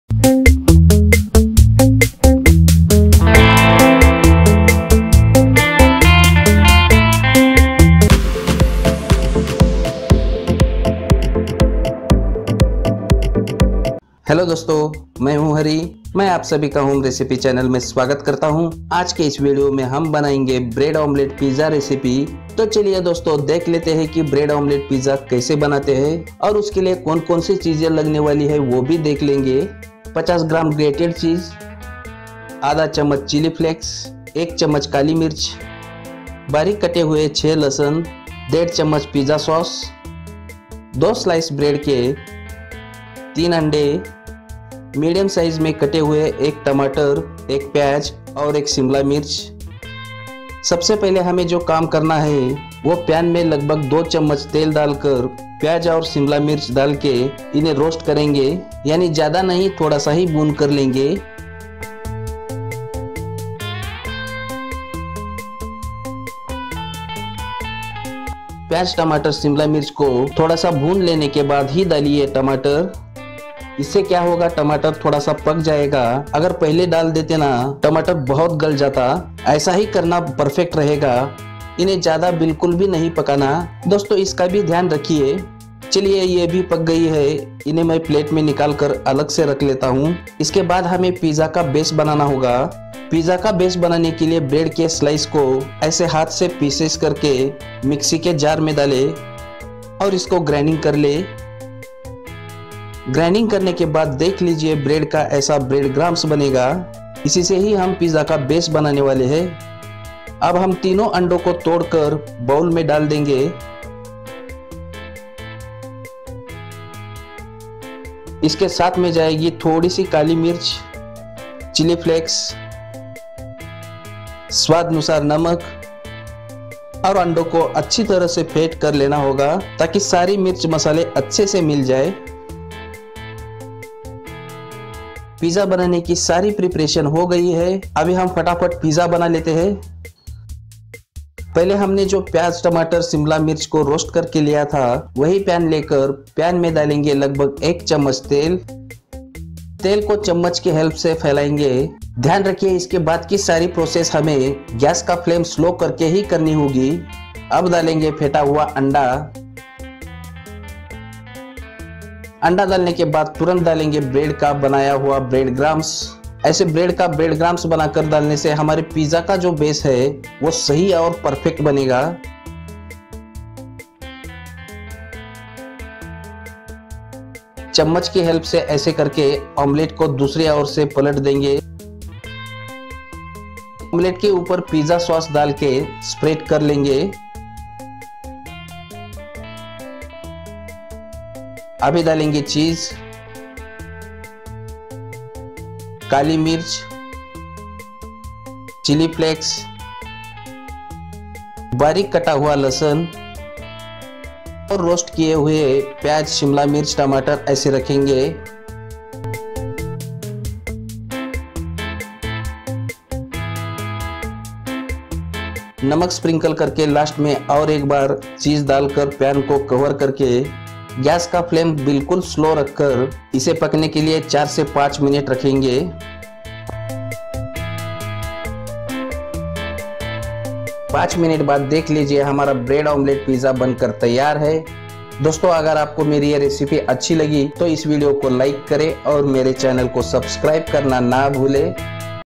Bread omelette pizza recipe || Easy and simple bread egg pizza recipe by home recipe
Easy and simple Bread egg omelette pizza recipe make at home